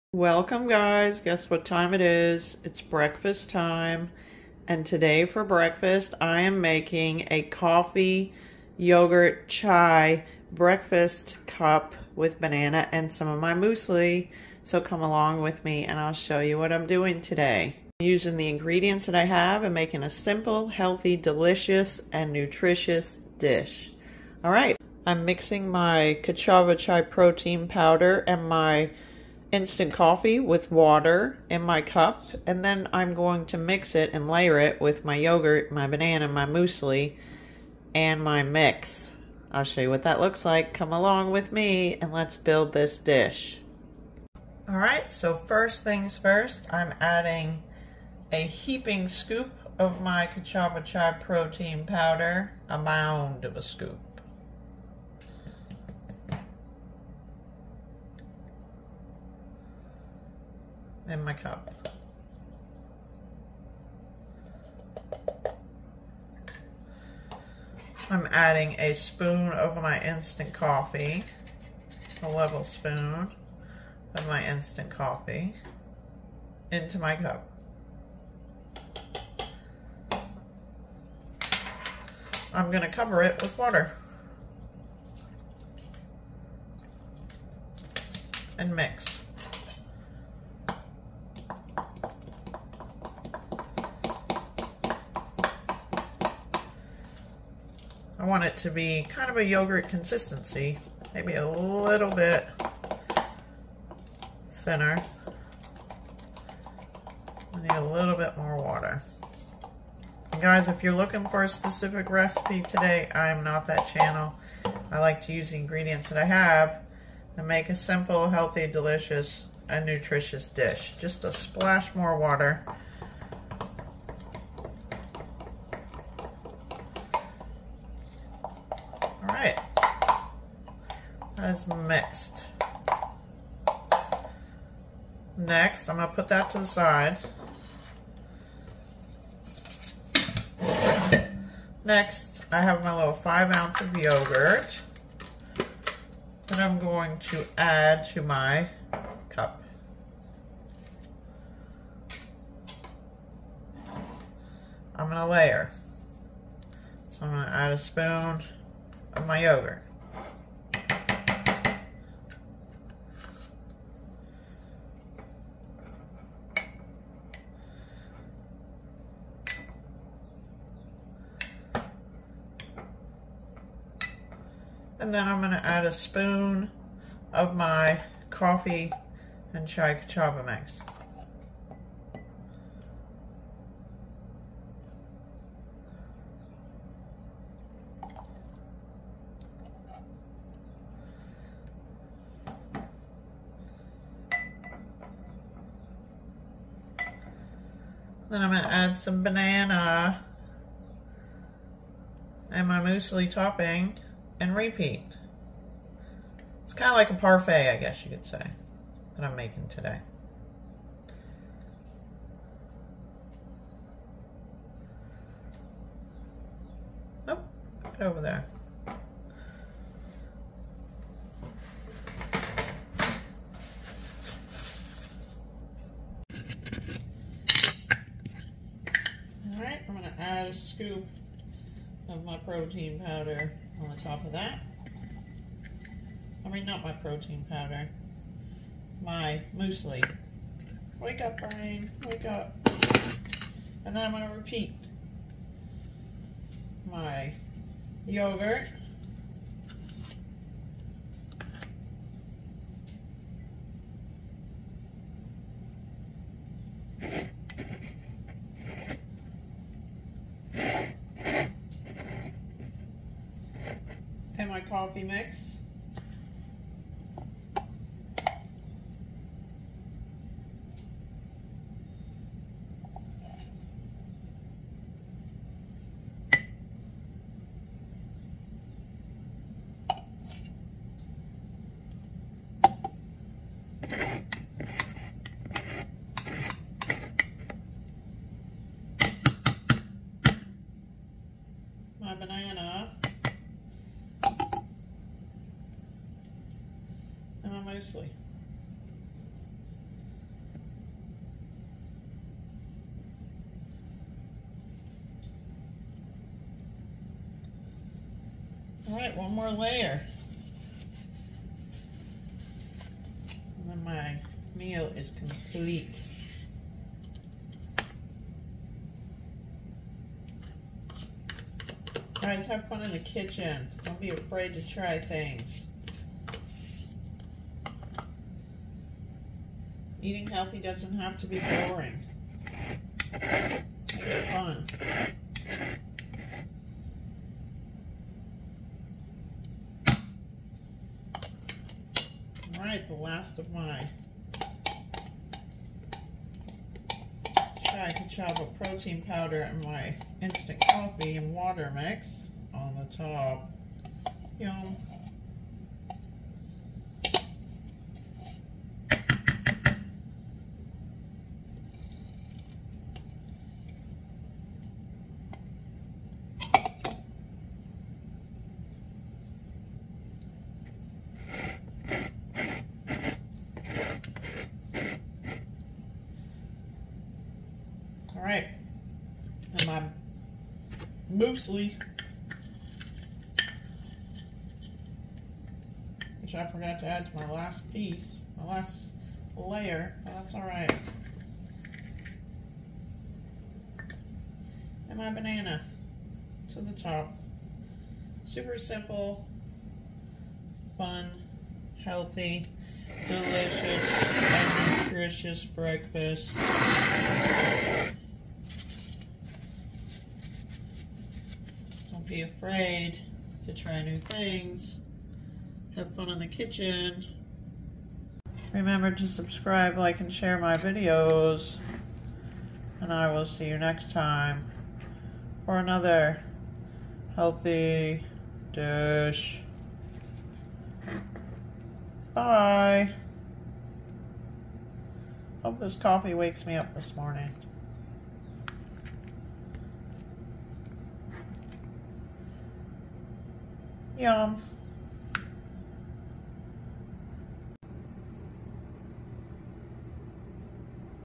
Breakfast today, Coffee Chai yogurt Parfait with Banana and Musilli. Yum! *Subscribe*
Breakfast today,  Coffee Chai yogurt Parfait with Banana and Musilli. Yum! Follow along to see how to make this delicious and nutritious dish.

Hey, get $15 off your first order at Ka'Chava